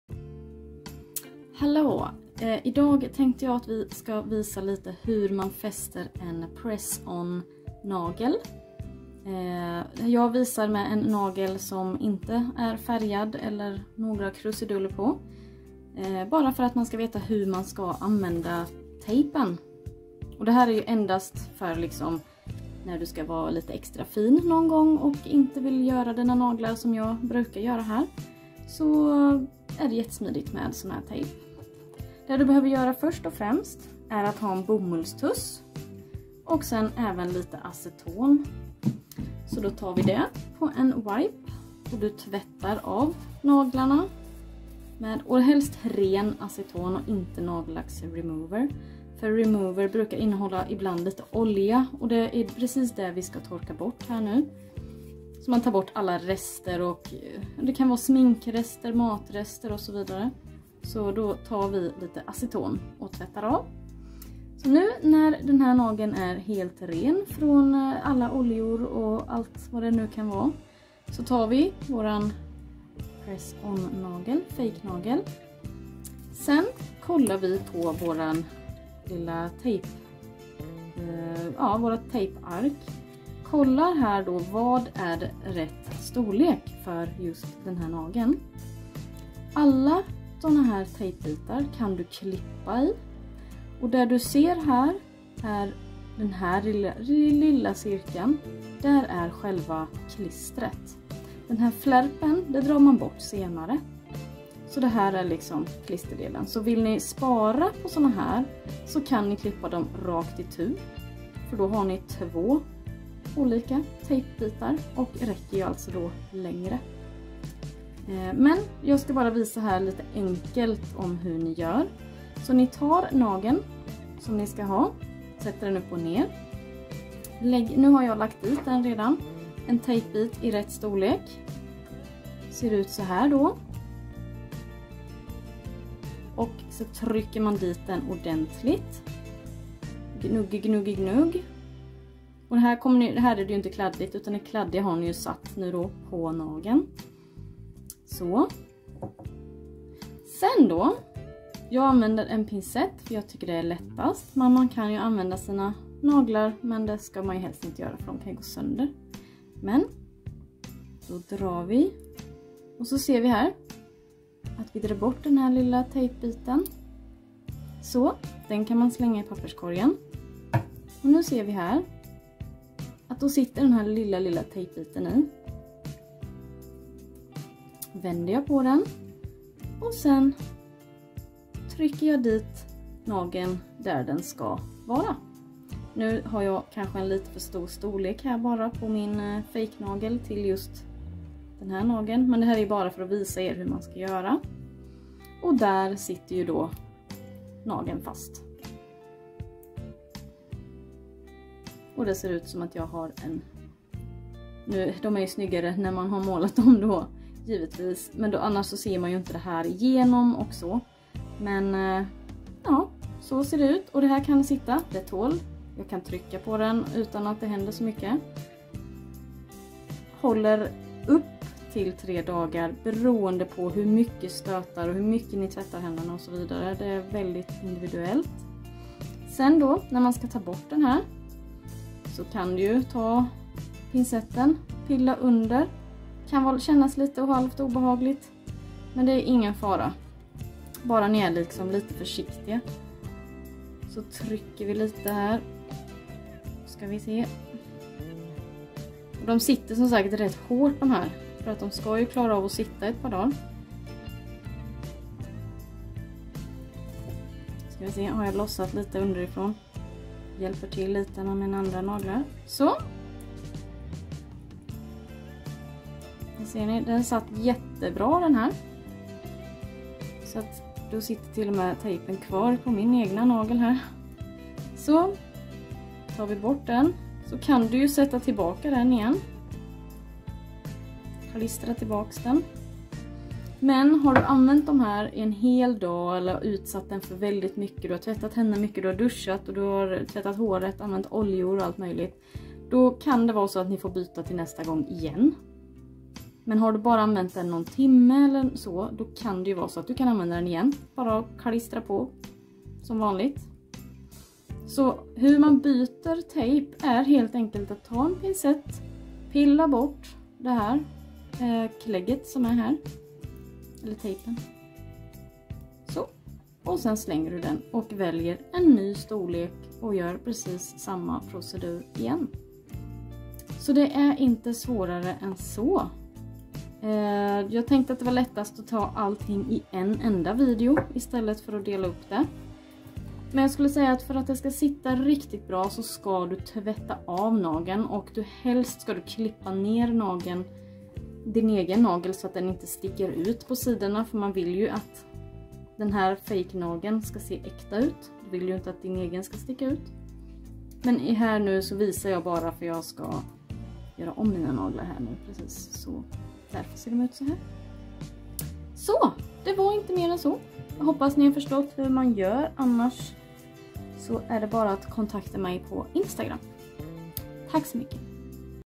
HOW TO USE GLUE TABS - PRESS ON NAILS | IN SWEDISH VOICE
Här kommer en video på hur du använder Nail Glue Tabs.

1. Tvätta av nageln med ren Aceton (gripens röda) med en bommullspad.
2. Klipp/fila ner din naturliga nagel.
3. Välj rätt storlek på din tejpbit (alternativt klipp alla tejpbitar på mitten men försök ta i rätt storlek som möjligt.)
4. Klistra på den på din naturliga nagel.
5. Va försiktig när du ska ta bort naglarna, det kan kännas lite obehagligt om de inte suttit på så länge, men det är inget farligt.
6. För att återanvända så kan du stätta tillbaka tejpbiten i fejknageln igen men om du haft dom på i flera dagar kan du behöva byta till en ny.